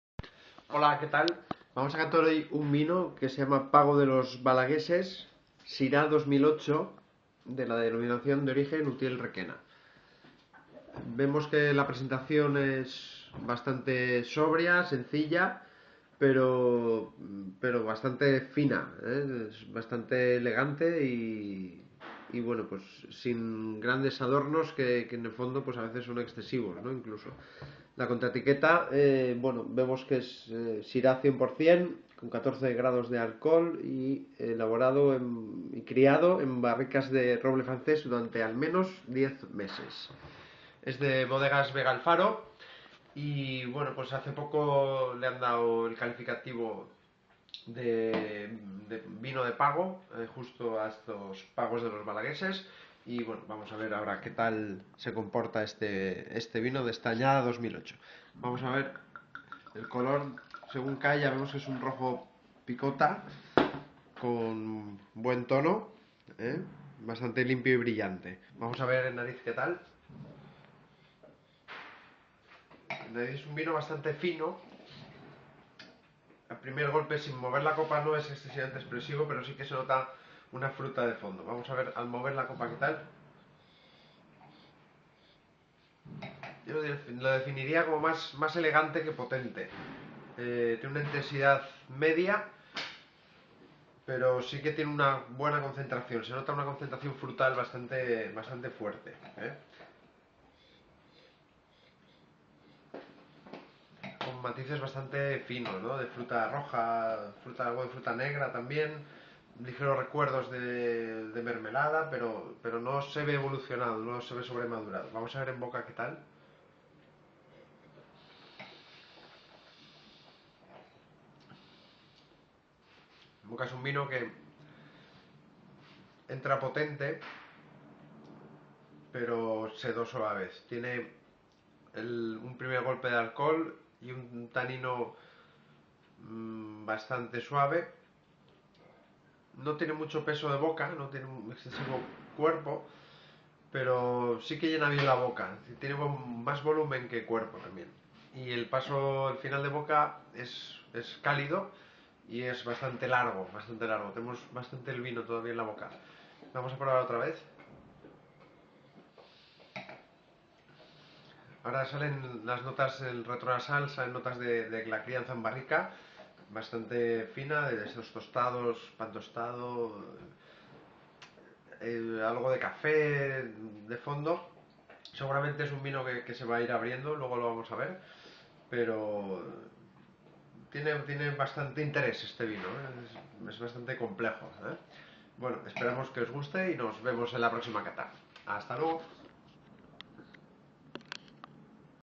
Pago de los Balagueses Syrah 2008
Cata de Pago de los Balagueses Syrah 2008. Un vino que sigue manteniendo una calidad asombrosa con los años. Se huele un terruño, se palpa un esfuerzo, se intuye una ilusión... y da como resultado uno de los vinos más emblemáticos de la Comunidad Valenciana, a veces no tan reconocido.